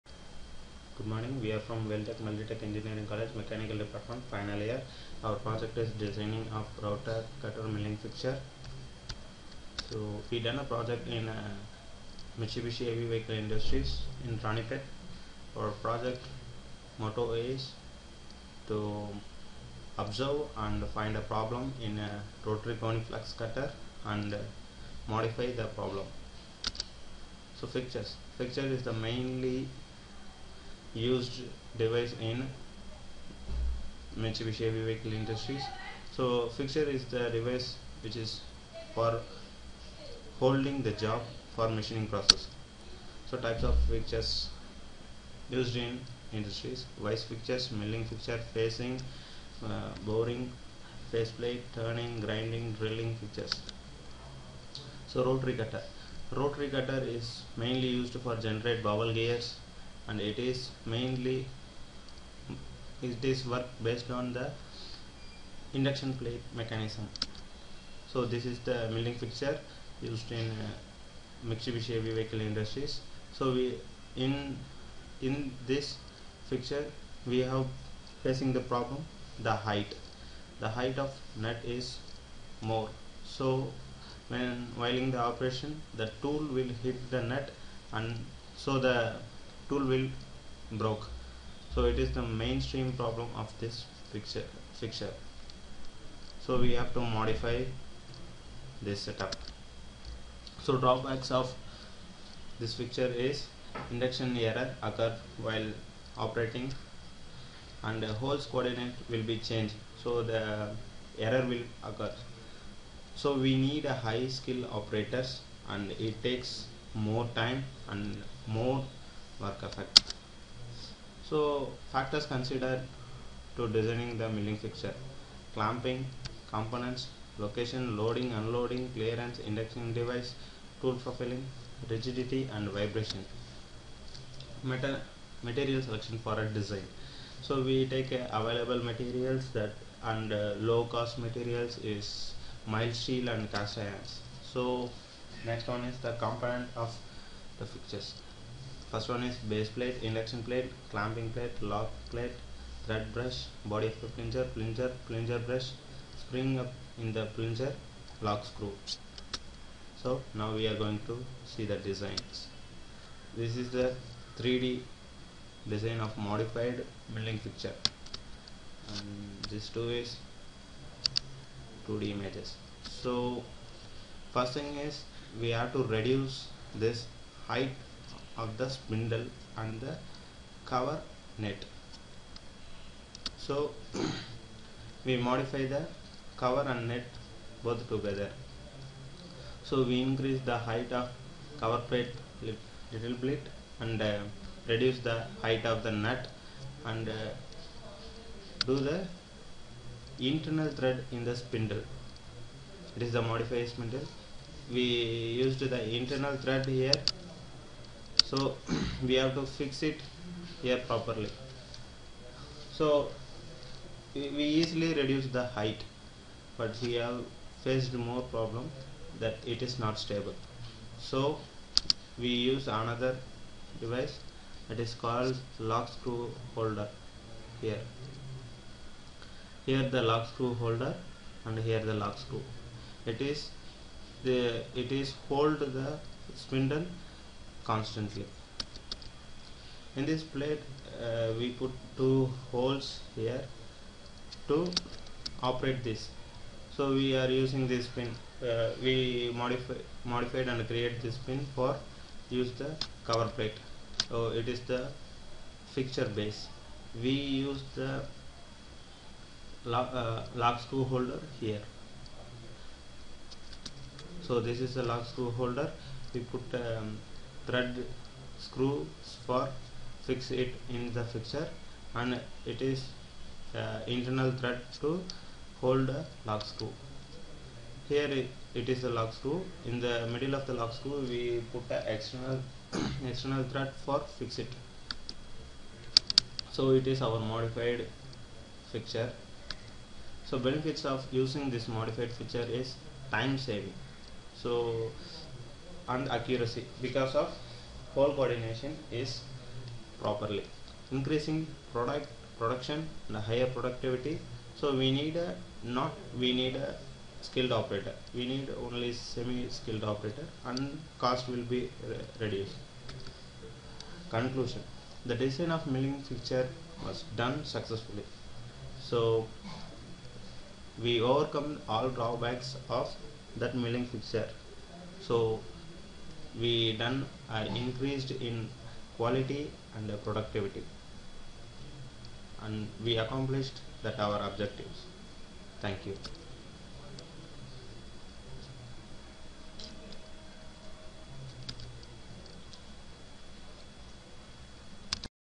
DESIGN OF ROTARY CUTTER MILLING FIXTURE
Final Year Project Presentation
By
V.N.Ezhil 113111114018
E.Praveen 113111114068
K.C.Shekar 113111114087
Of
Vel Tech Multi Tech Engg College,
Avadi.